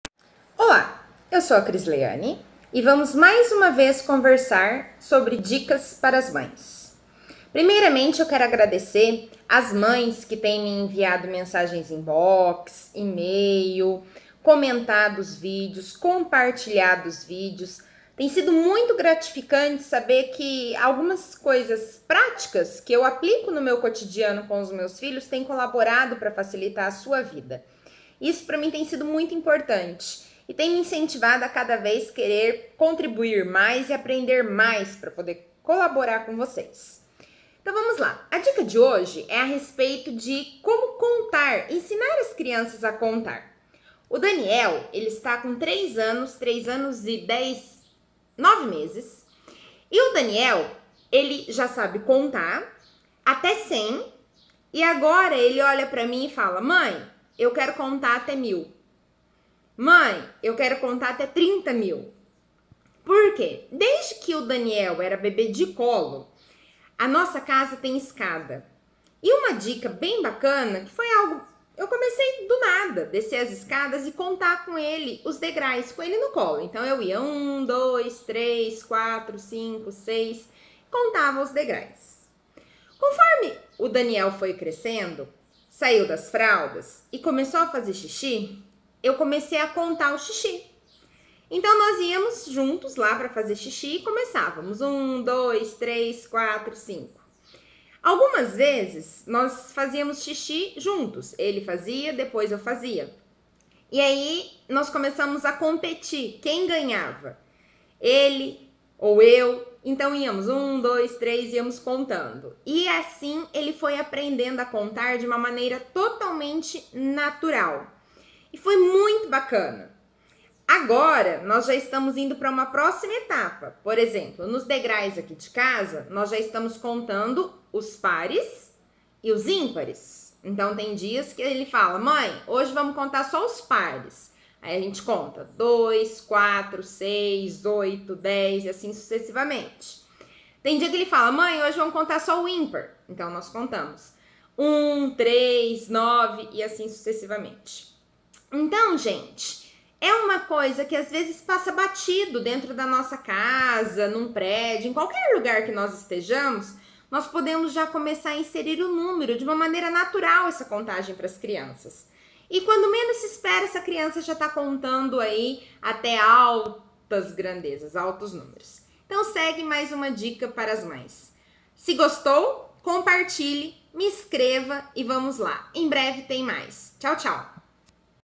COMO ENSINAR MEU FILHO a CONTAR - MANEIRA PRÁTICA DE ENSINAR O SEU FILHO. ENSINE BRINCANDO
Como ensinar meu filho a contar: Dicas práticas para ensinar seu filho a contar desde bem cedo..